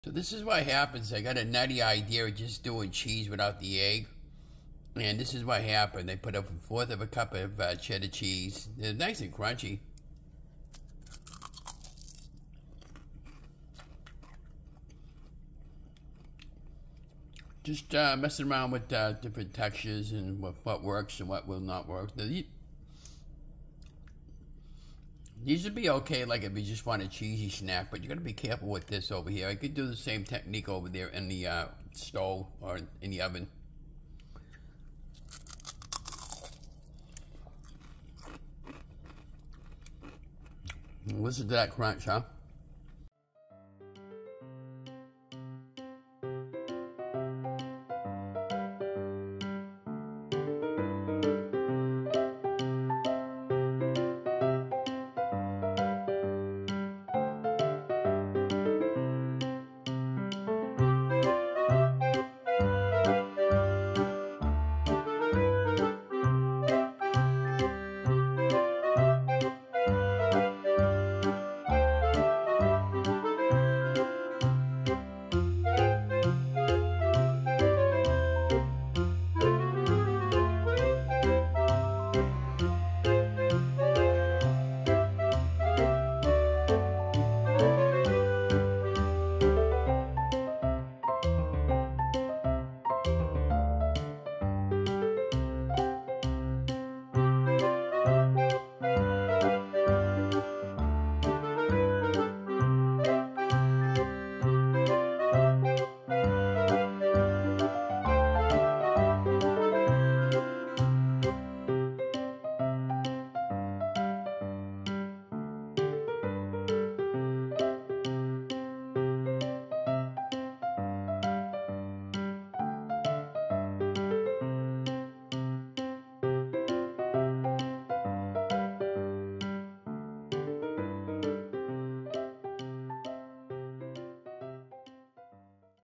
Chaffle 4 Life Episode 33 Simple Crunchy No Egg Cheddar Cheese Chaffle! Ron’s Keto Café…
Chaffle 4 Life Episode 33 Simple Crunchy No Egg Cheddar Cheese Chaffle! Ron’s Keto Café… ¼ cup Cheddar Cheese or cheese of your choice. Be careful not to burn yourself when pulling the Chaffle off of your Mini Dash! These came out nice and crunchy. I have been experimenting.
Ron’s Keto Café Facebook Support Group